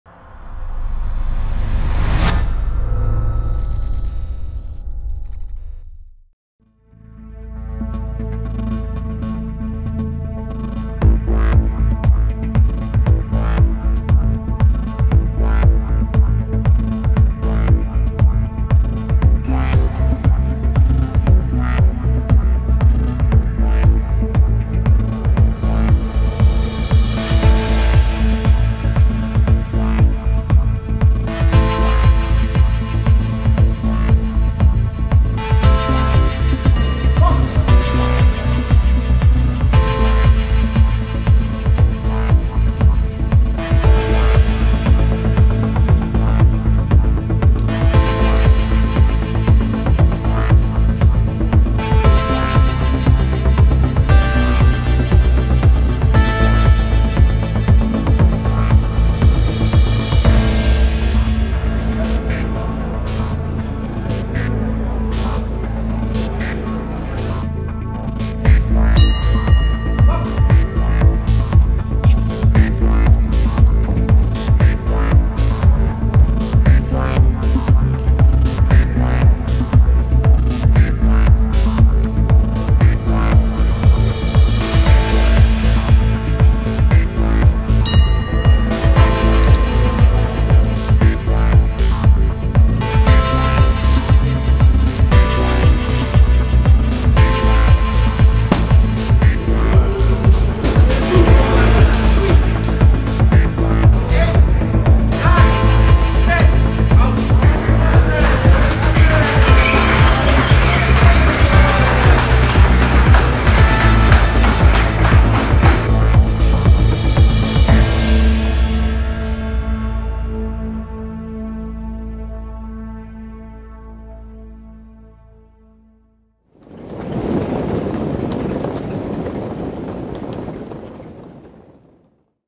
Jose Antonio Sanchez Romero vs Maksims Danis - 14.5.2022 Ringside Gym
Rayito facing Maksims Danis (highlights)